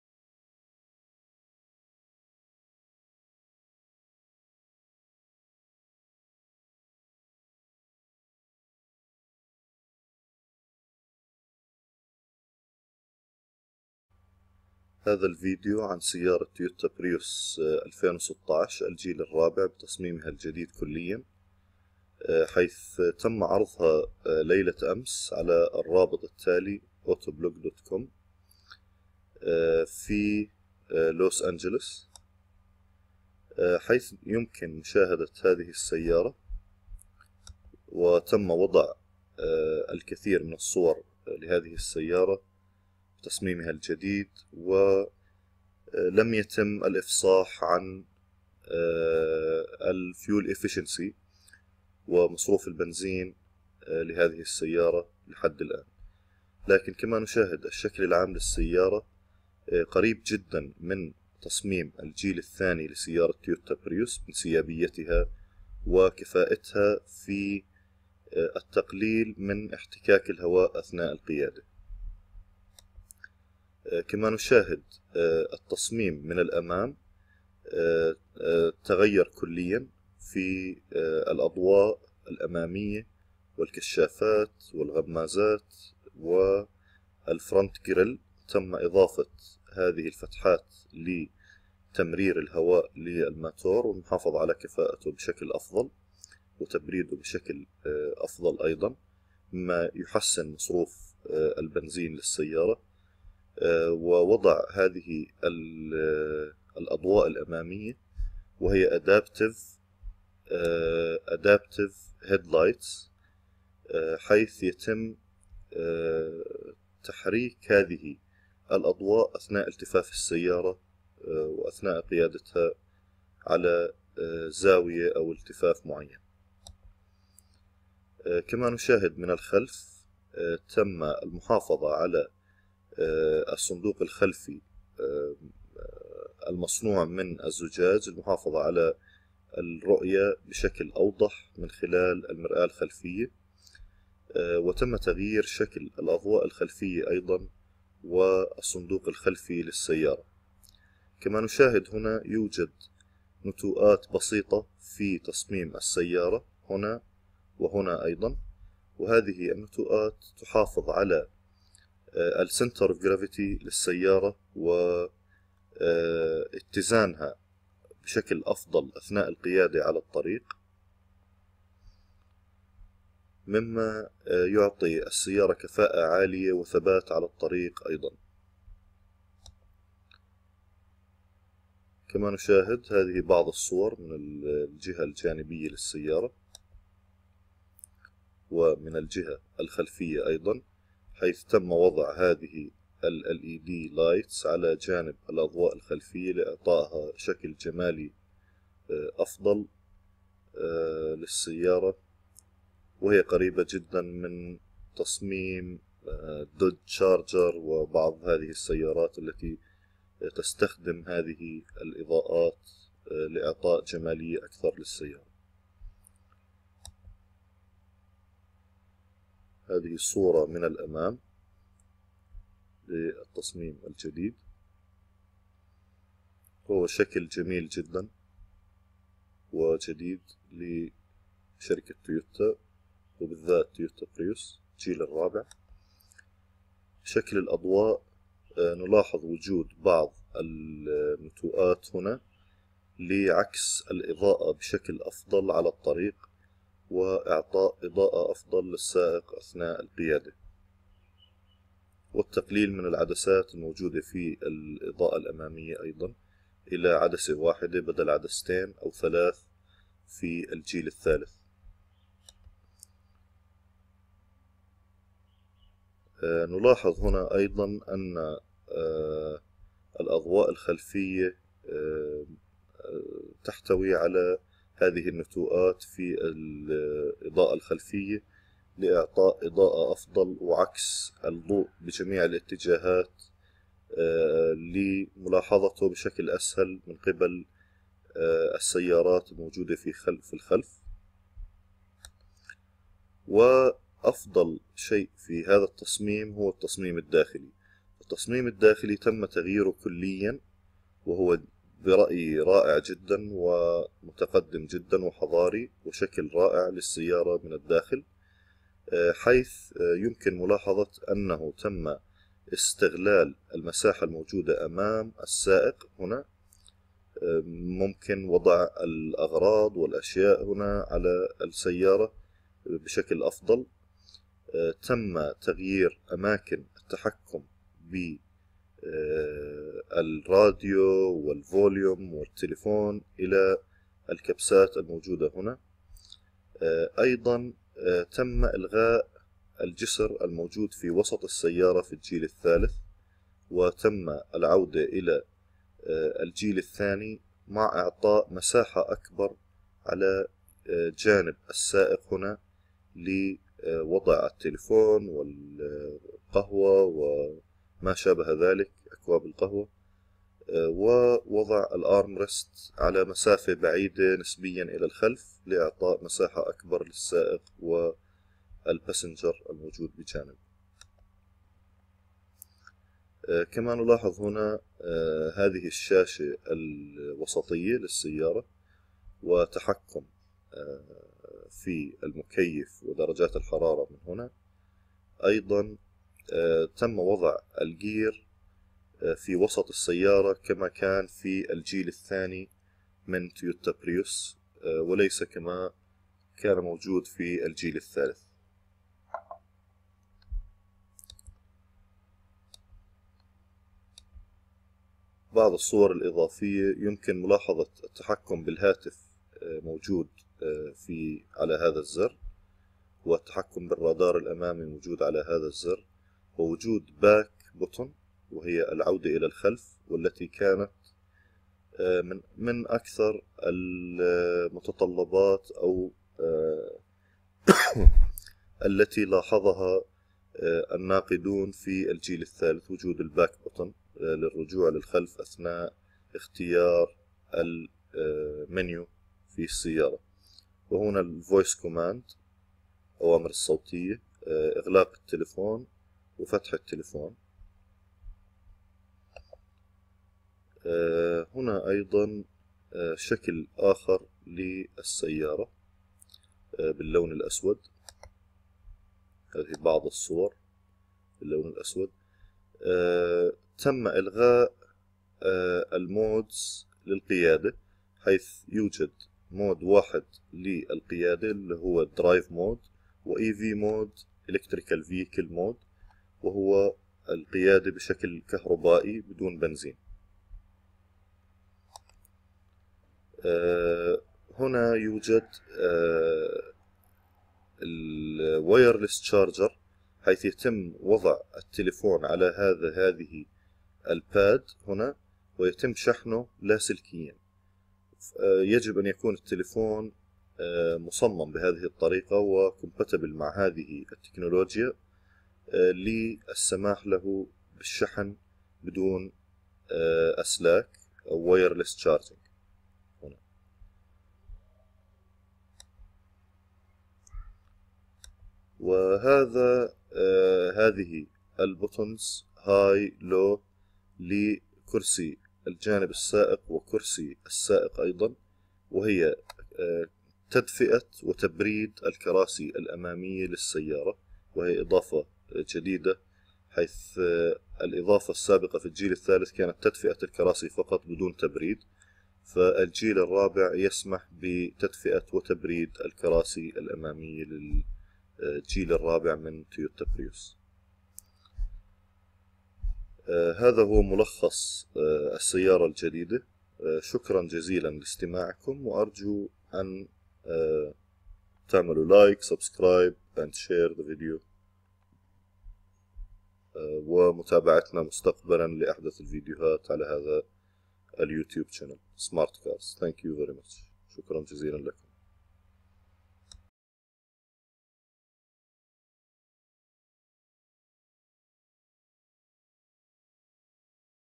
تويوتا بريوس 2016 الجيل الرابع هايبرد هجينه (تم عرضها رسميا في لاس فيجاس)
سيارة تويوتا بريوس 2016 الجيل الرابع تم عرضها في لاس فيجاس 9-9-2015 بتصميم جديد كلياً واضافات جديدة من تويوتا بريوس هايبرد الهجينة الجيل الرابع 2016